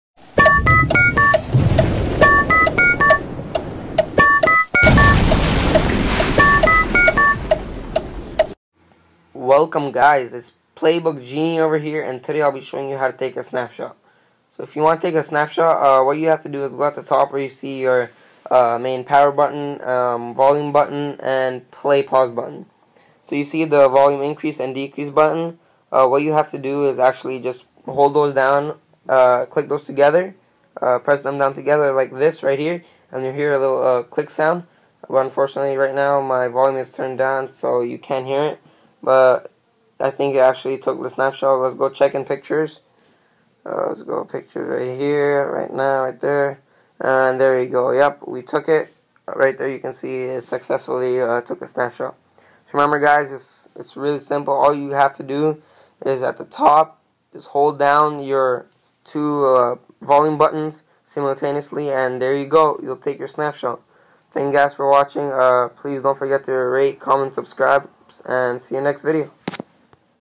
Blackberry Playbook Tutorial - How to take screenshot
Blackberry Playbook Tutorial - How to take screenshot-Welcome back guys in this video I show you how to take a screenshot on your playbook. As i explained all you have to do is hold down the two volume buttons together at the same time and Voila! So its as easy as that. Thank you guys for watching, if you have any questions or concerns please write it down in the comments and ill reply as soon as possible.
TAGS " How to", " How to take" , "blackberry playbook".